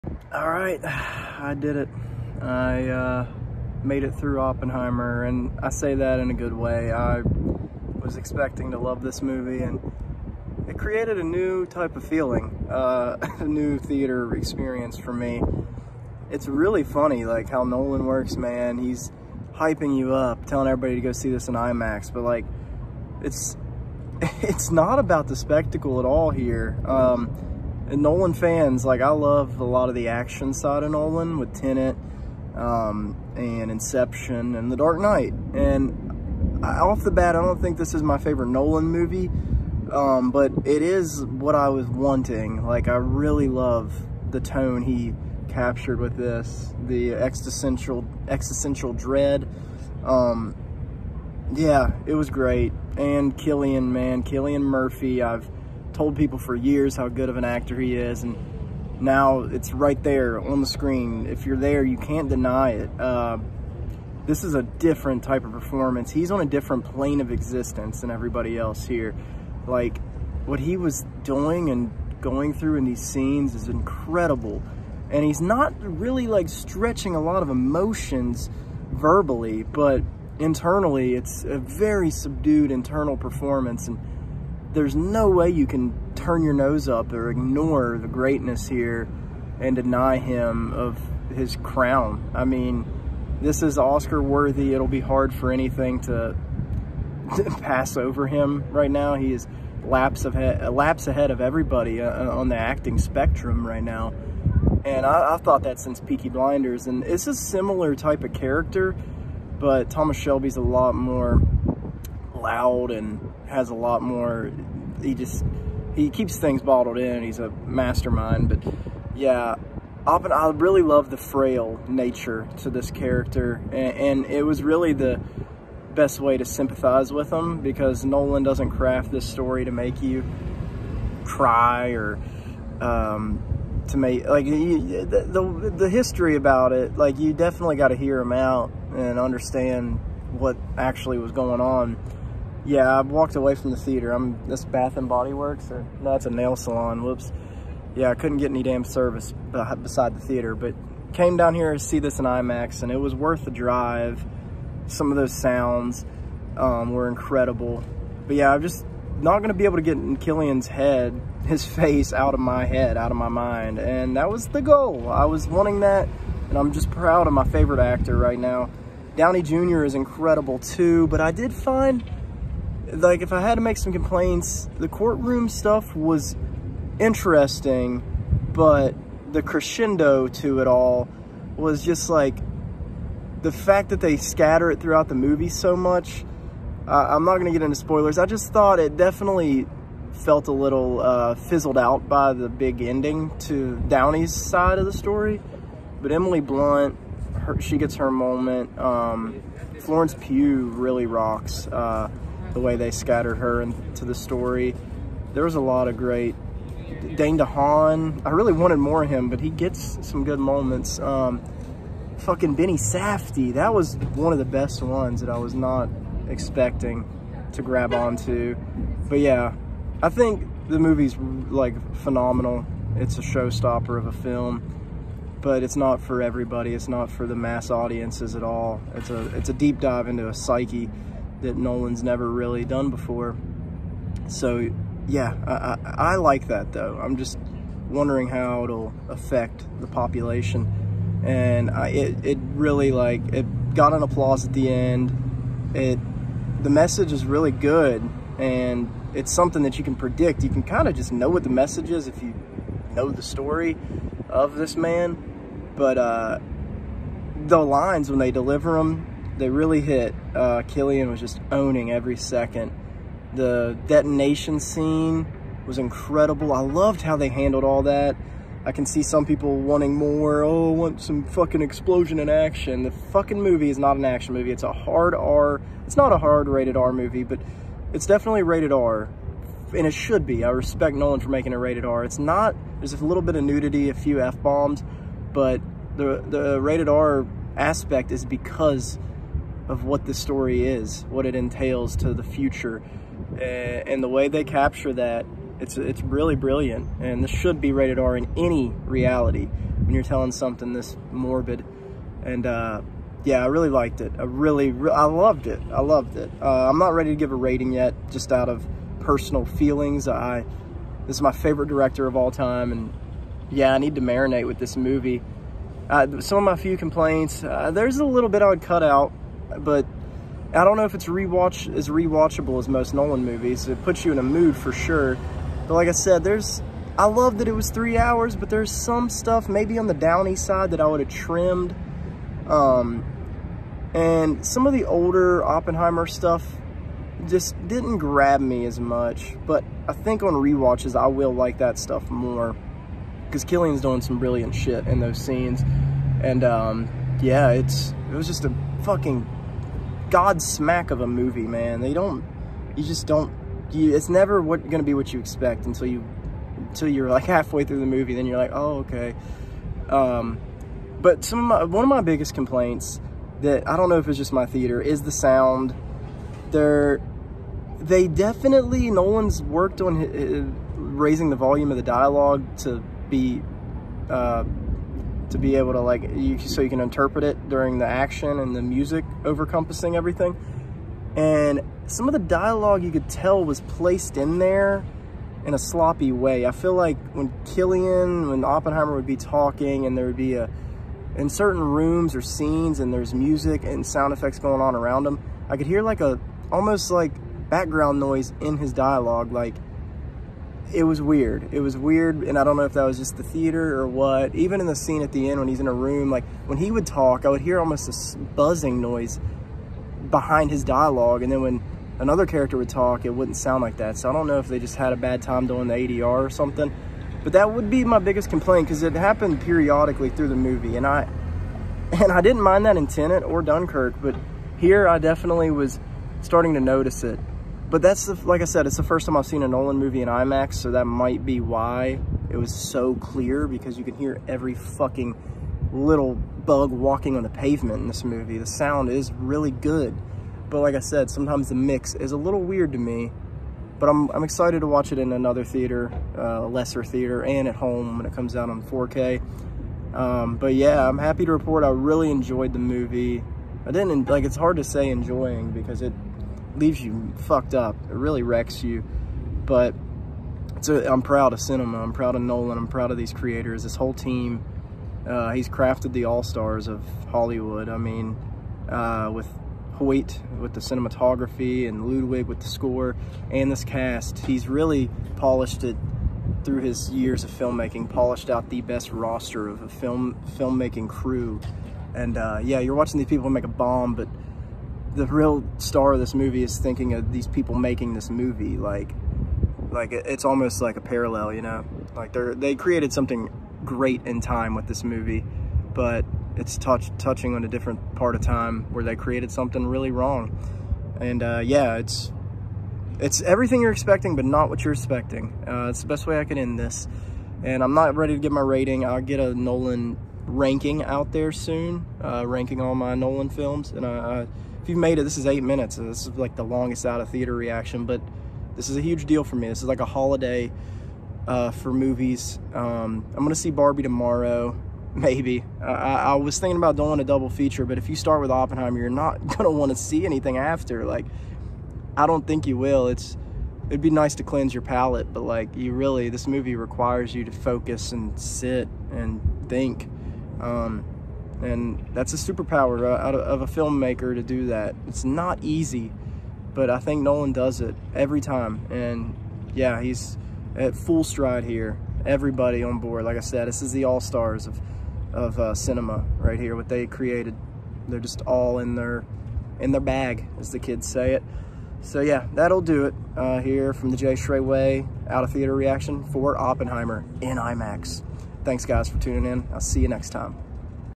Oppenheimer (2023) OUT OF THEATER REACTION/ REVIEW! | IMAX | Cillian Murphy | Christopher Nolan
We made it Nolan fans! Here are my thoughts on my most anticipated movie this year!